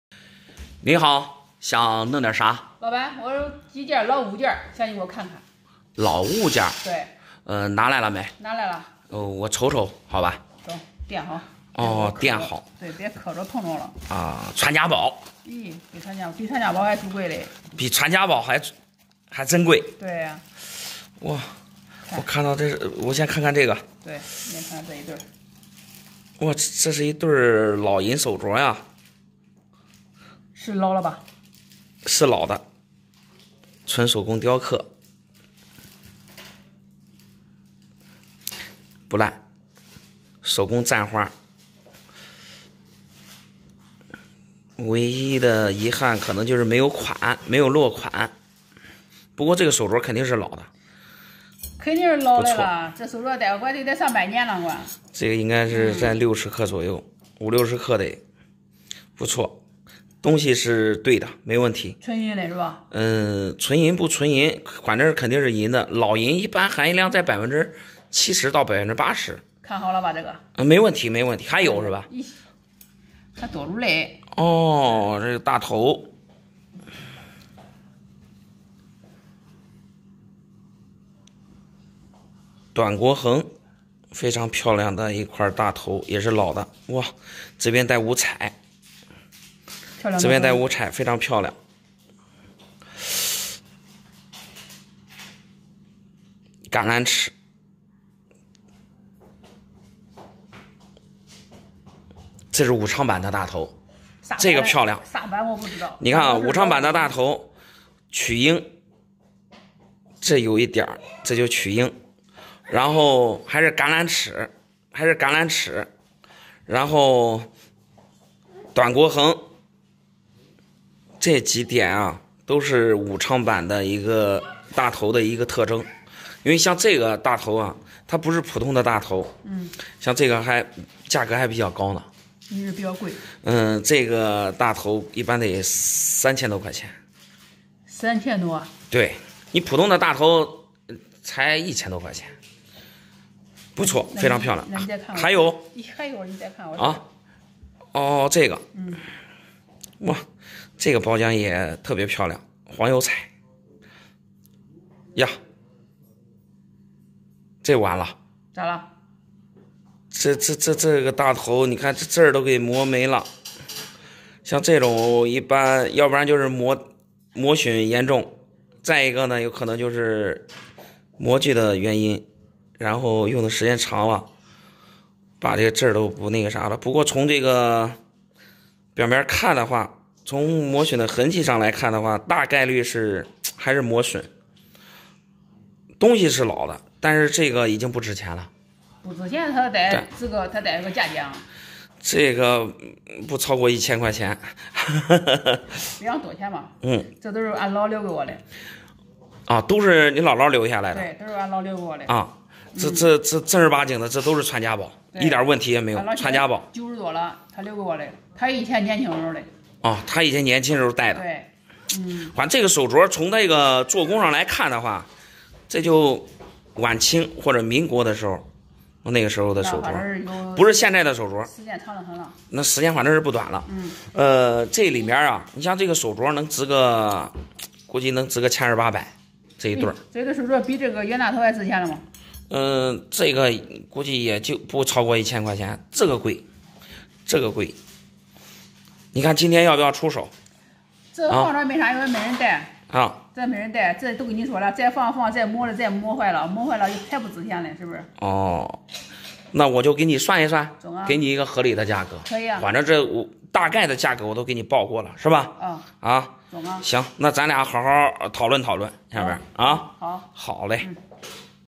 姥姥给的传家宝银圆，里面还有个值钱的特殊版别，看能估价多少？【小闫打铁镯】
请放心，本视频是一部原创拍摄的视频，没有任何不良行为。 女子拿姥姥留下的传家宝找小闫鉴定，有银圆，银手镯，小闫一看有个特别值钱的版别！